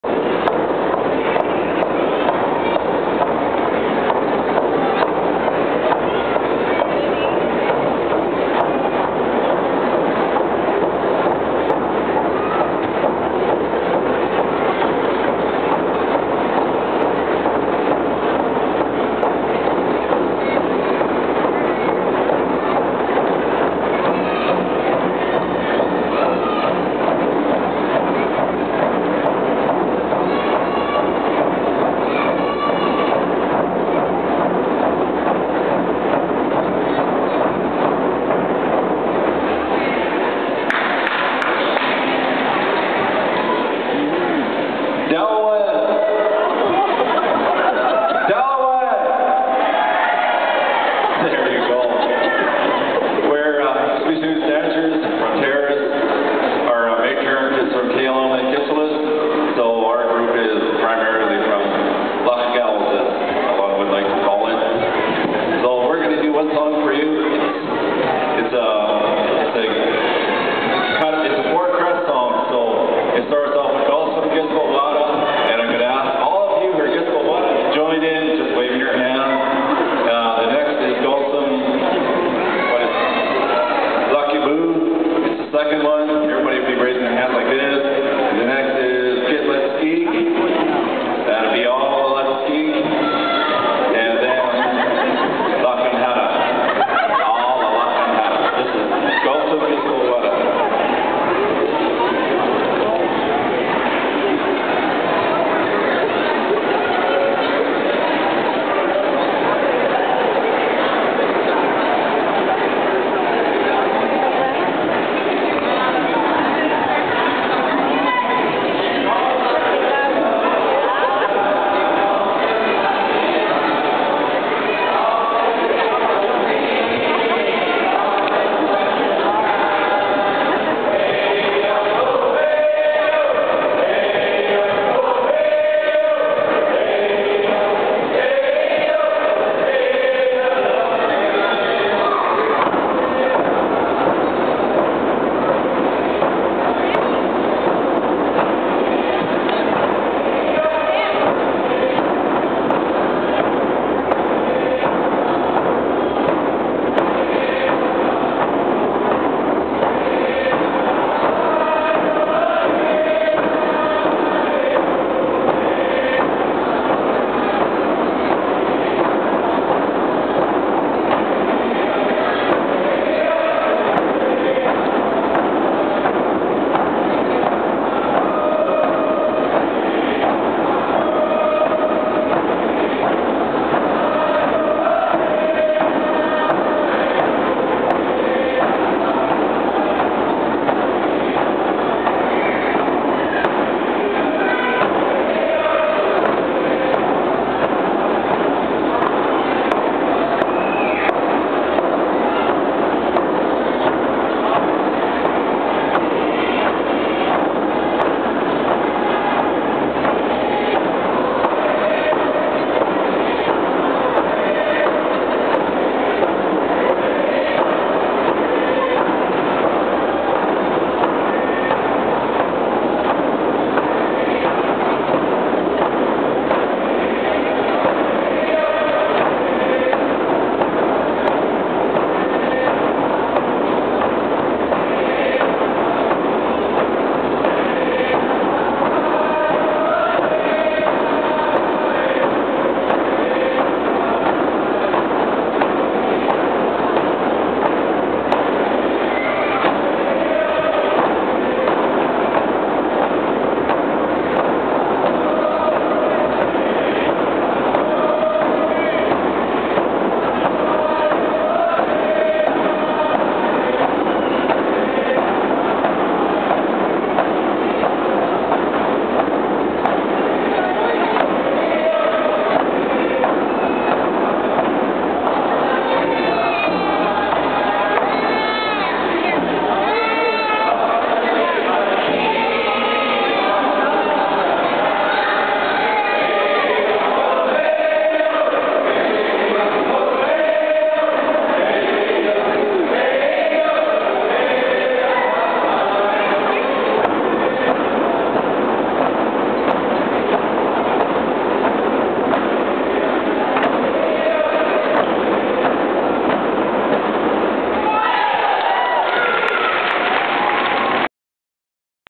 Terrace Group Xbishuunts Dancers Tsimshian Nation
Xbishuundts Dancers-Tsimshian Nation Culture day performance in Prince Rupert, B.C. - Feb 6, 2009 at for the 50th ANBT.